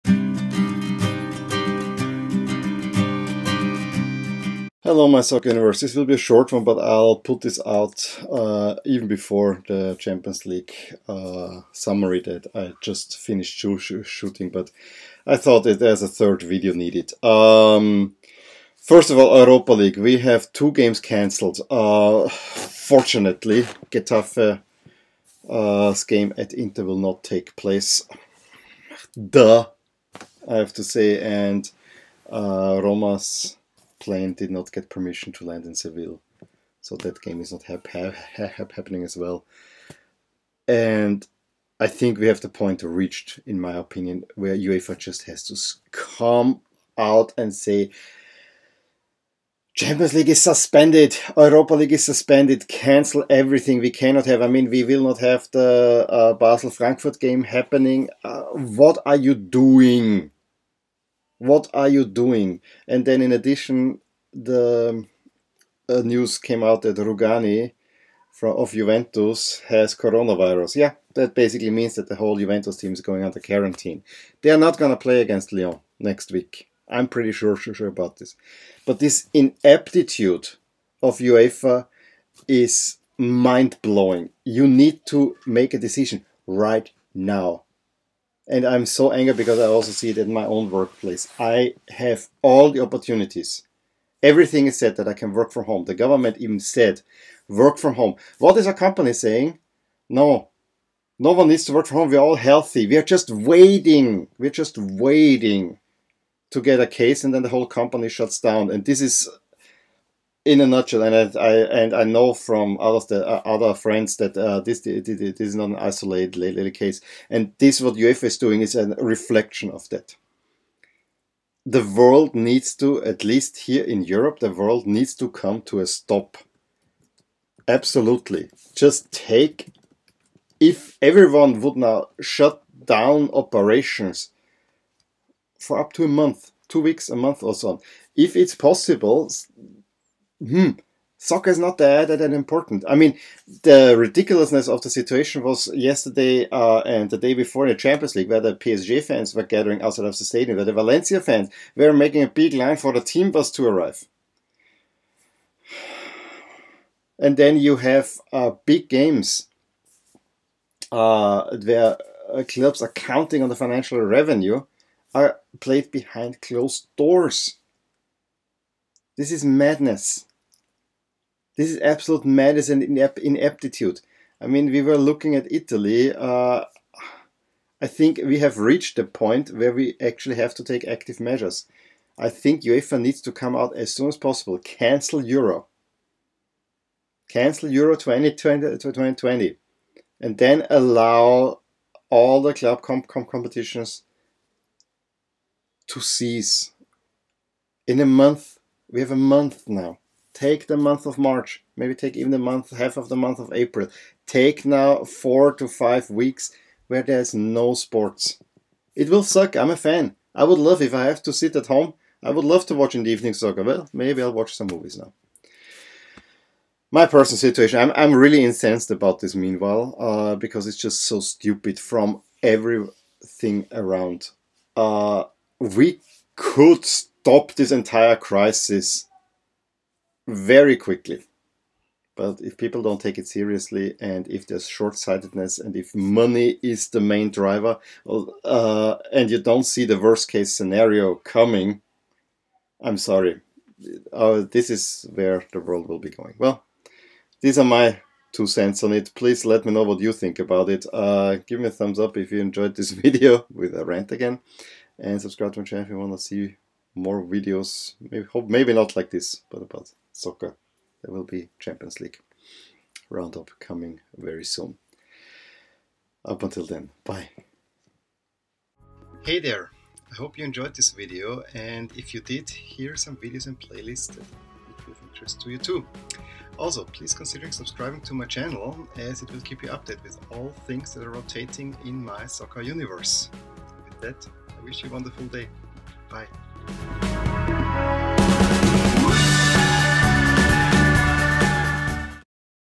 More Corona Madness: How UEFA's Inadequacy is a Reflection of Society
In the face of match cancellations in the Europa League, Fans gathering outside of the stadiums and even players contracting the virus, one has to seriously ask: when is UEFA finally pulling the plug??? But then, it is just a reflection of society ...